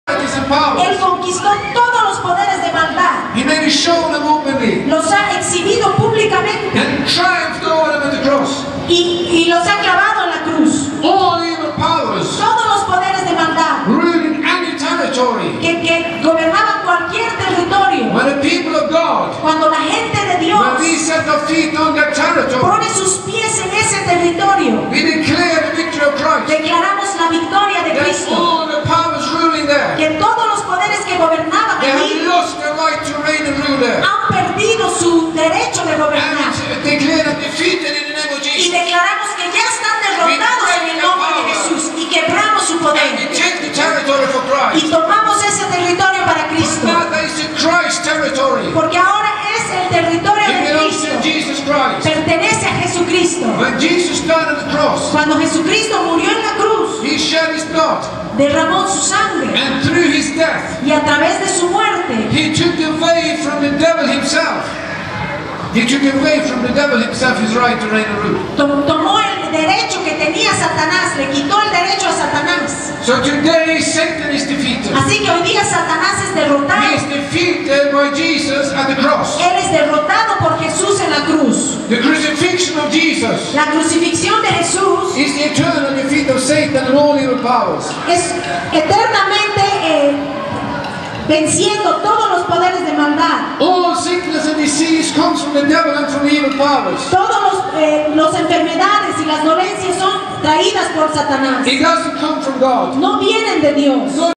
Svein Preaching with power
More preaching by Svein from Punata 2013, Preching theoriginal, raw, New Testament gospel with power and authority!